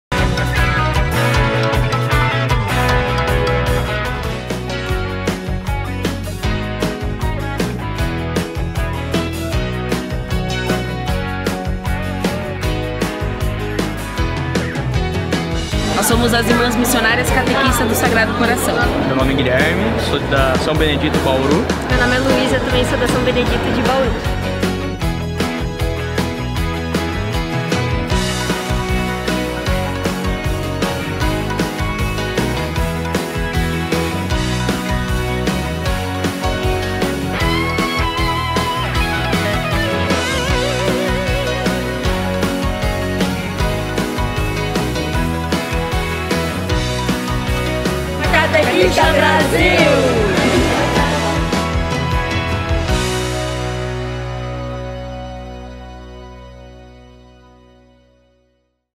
Bem-vindo(a) ao Catequistas Brasil 2019!
Assista a algumas cenas do primeiro dia deste grande evento que foi realizado do dia 08 ao dia 10/fev de 2019.